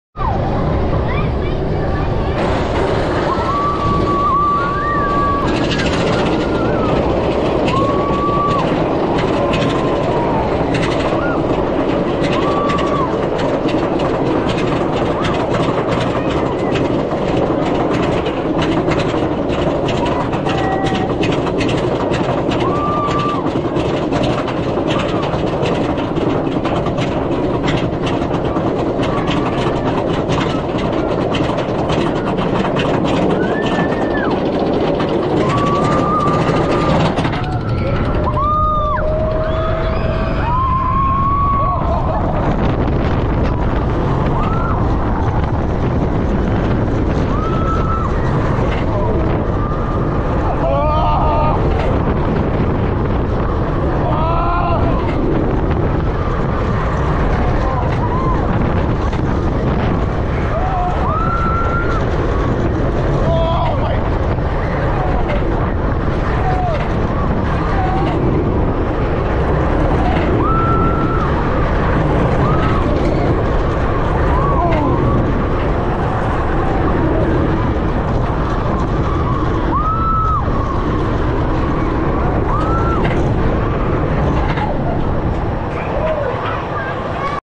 Twisted Timbers POV 4K 60fps Horizon Leveled BACKWARDS Kings Dominion Doswell, VA
travel to TwistedTimbers is an elite roller coaster made by RockyMountainConstruction as in considered by many to one of their best creations located at kingsdominion This is the conversion of the former Hurler wooden roller coaster and it could not be better! The slight theme and antics of the ride will keep you wanting more! Providing strong ejector airtime and great laterals. Height:110ft MPH:54